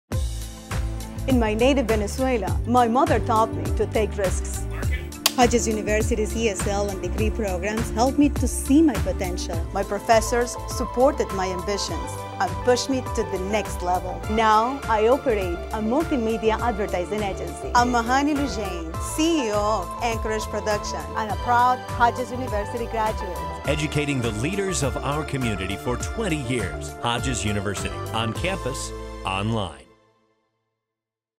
Dreamtime Entertainment Commercial Hodges University
Commercial for our client Hodges University featuring Majani Lullein. an ESL student and business owner.
DP: Flip Minott, John Paluzzi
Directed by Dave Beaty
Edited By Dave Beaty